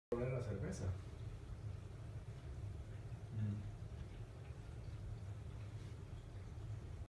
My Cat Smells Beer
Tiger smells Budweiser Black.